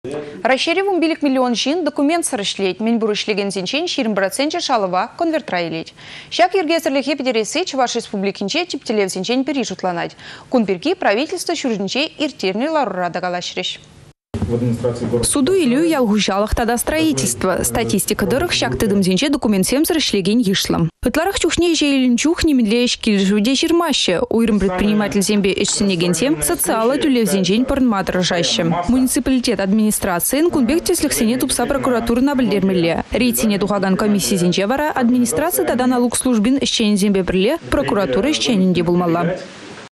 Документсăр ĕçлекенсемпе кĕрешмелле
Раççейре 15 миллион çын документсăр ĕçлет. Мĕн пур  ĕçлекенсенчен 20 проценчĕ шалăва конвертра илет. Çак йĕркесĕрлĕхе пĕтересси Чăваш Республикинче тĕп тĕллевсенчен пĕри. Кун пирки  правительство çуртĕнче ирттернĕ ларура та калаçрĕç.